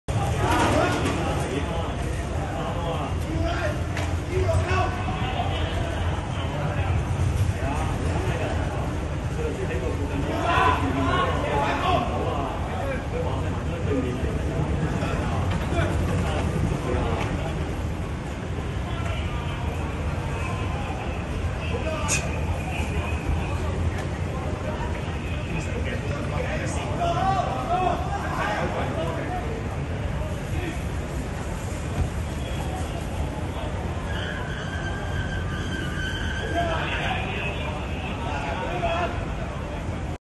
淘大花園商場有男子持刀遊蕩 警員接報到場制服
九龍灣淘大花園今日(5日)傍晚6時許，有男子持刀及推著手推車遊蕩，途人隨即報警。大批穿上防刺背心的警員接報到場，在一間快餐店門外發現該名男子，合力將對方制服，以涉嫌藏有攻擊性武器拘捕該名男子。男子被送往聯合醫院檢查，事件中無人受傷。警方正調查事件及他持刀的動機。

bmgproductionmusic